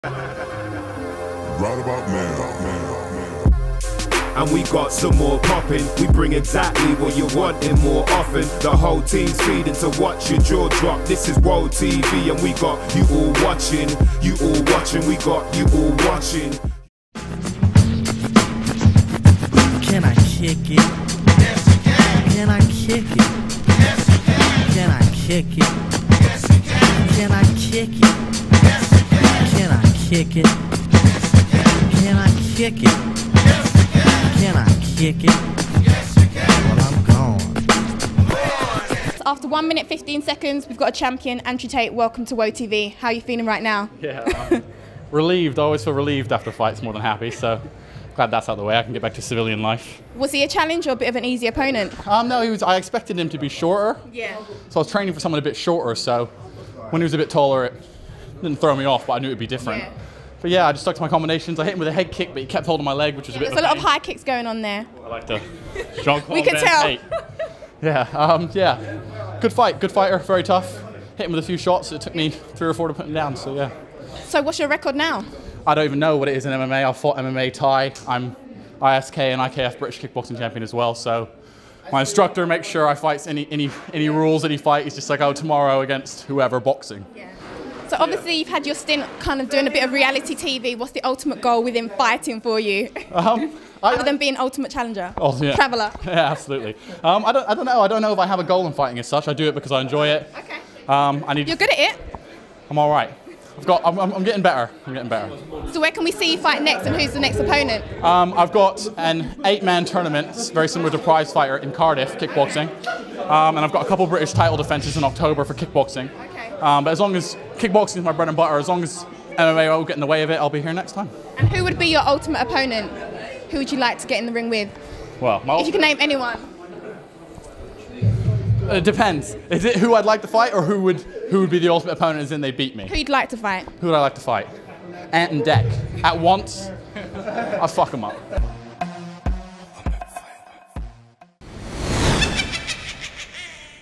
UWC - Andrew Tate
Andrew Tate speaks to WHOA! TV's Josanne Cobham